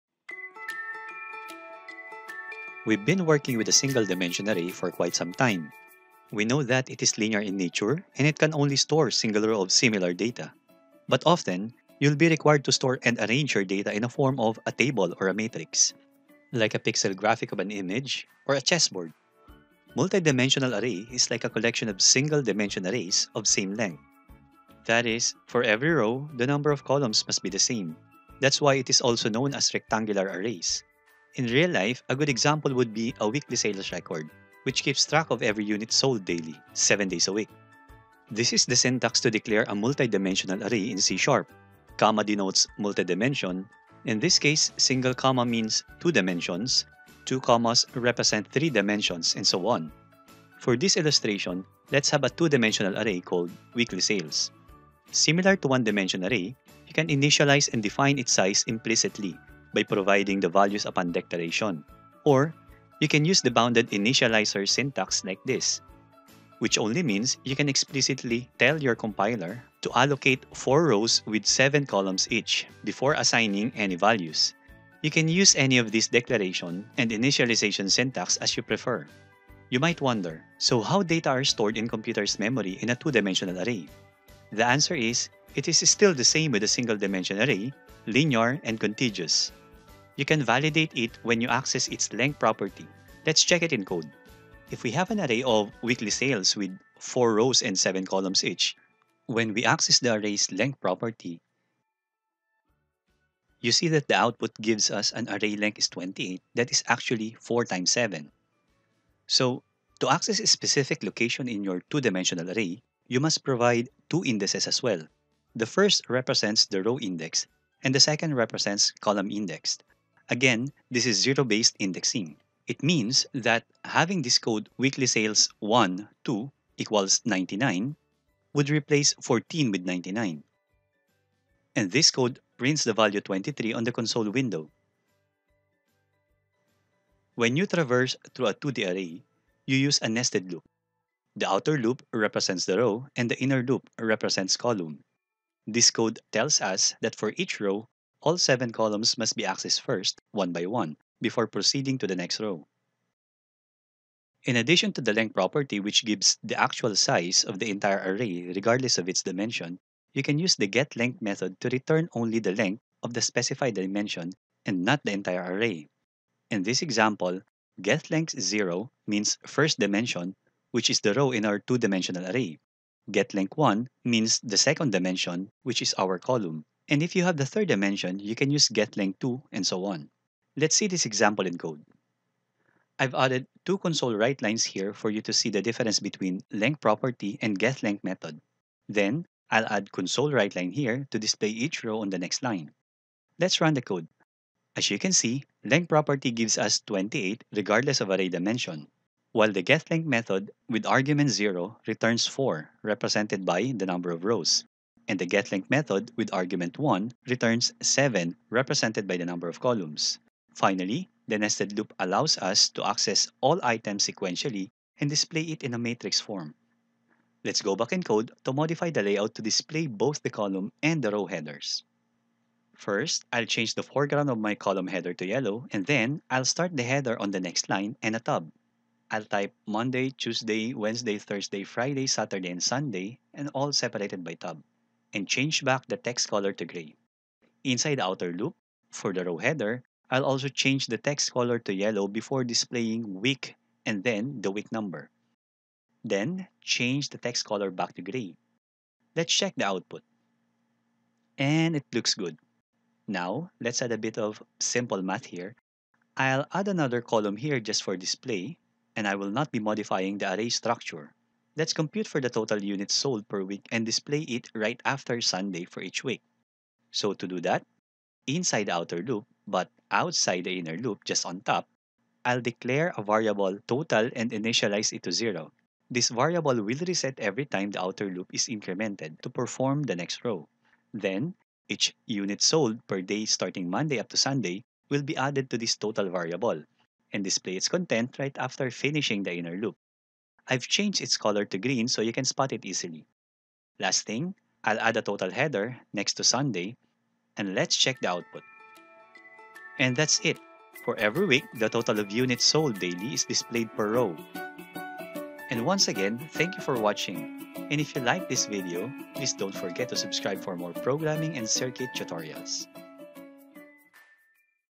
11 Multi-dimensional Array : Data Structures and Algorithms
In this video, Joed Goh explains how to work with multi-dimensional arrays in C#. The discussion highlights how to declare, initialize, access elements, and traverse in a two-dimensional (2D) array structure.

Additional topics to be updated soon...